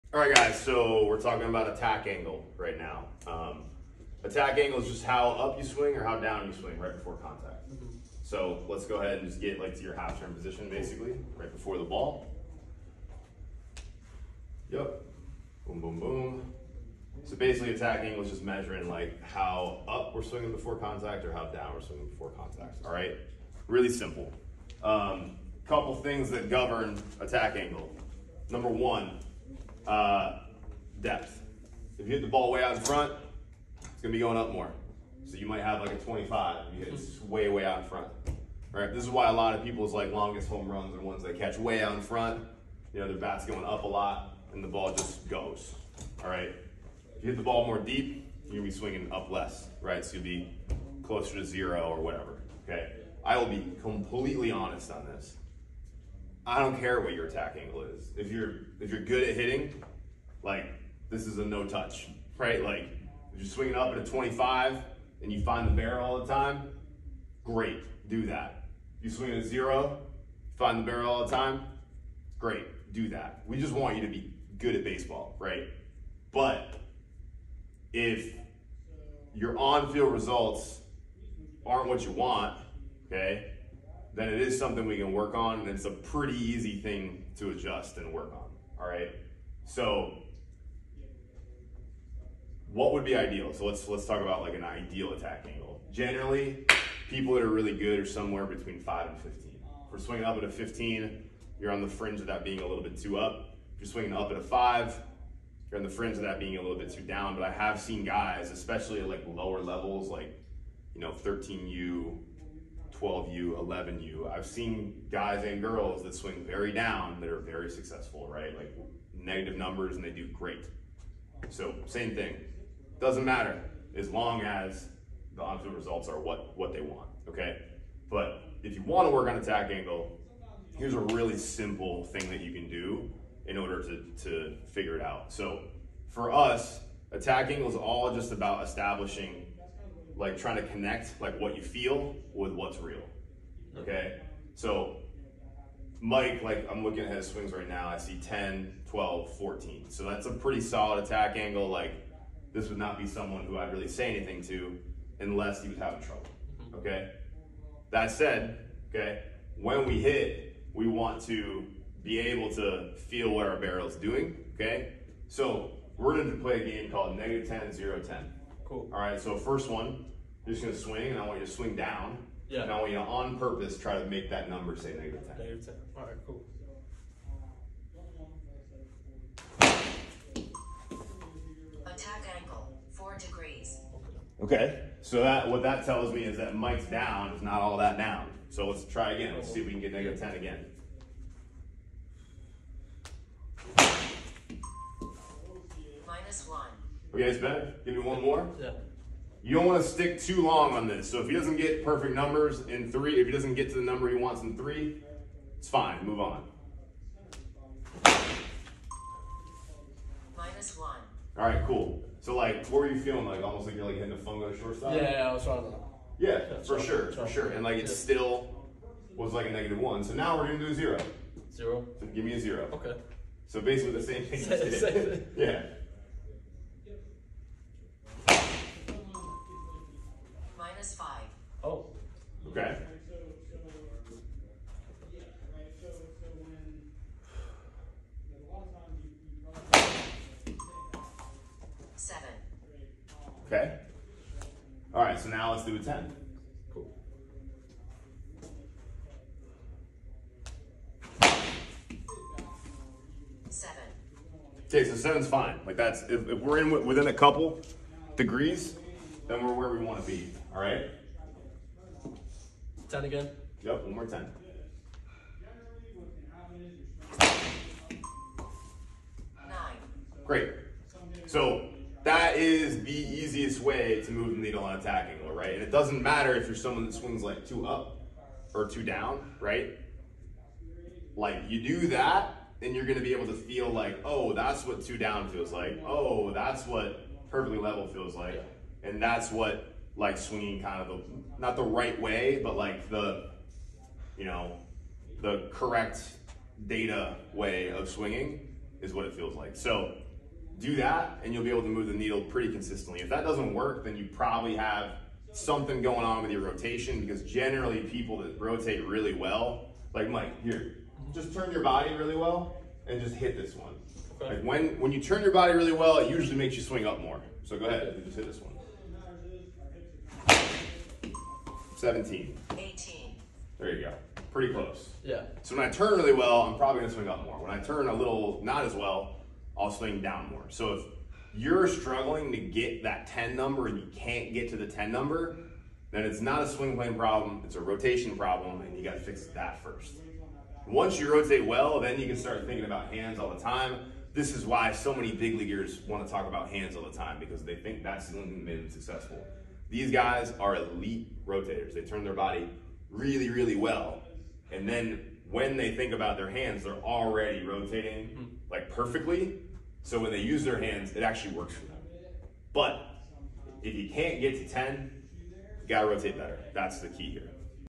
Attack Angle - Blast Motion
Attack angle measures how up or down the bat is going prior to contact.

A lot of things can affect your attack angle:
- your posture
- the depth of contact
- your hand path
- and many other things not mentioned

Generally, hitters want to be between 5 and 15 degrees. However, if the hitter is successful there is no need to make an adjustment.